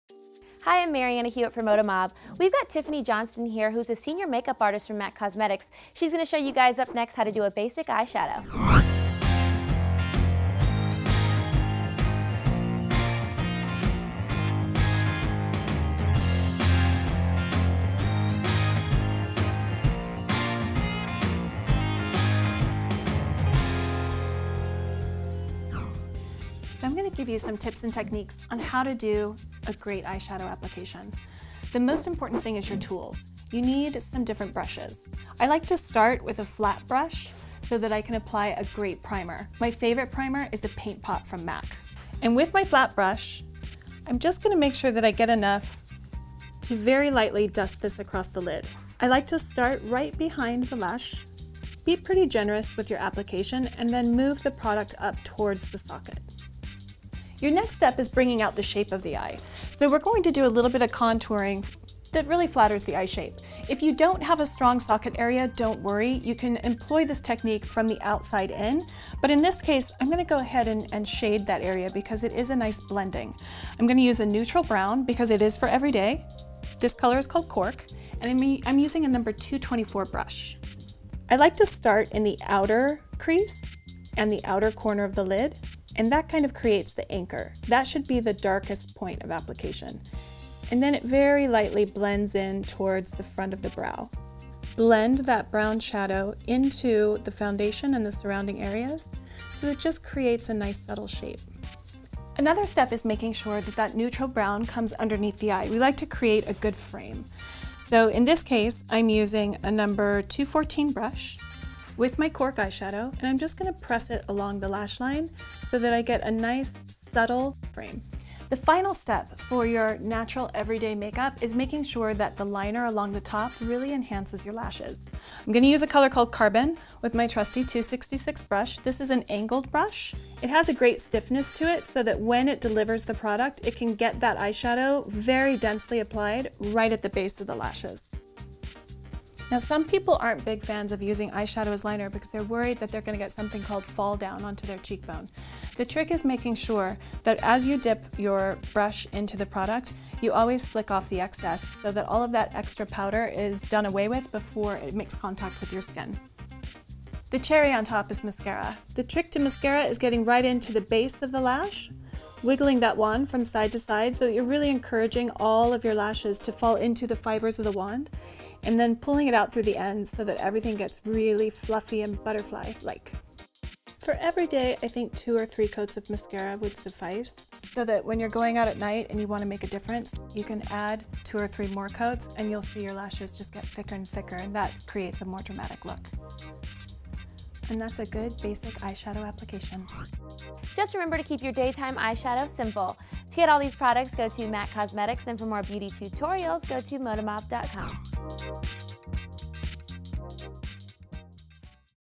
How To Do A Basic Eyeshadow Application
Marianna Hewitt and MAC Cosmetics Senior Makeup Artist Tiffany Johnston will take you step by step through how to do a simple and basic eyeshadow application like a pro.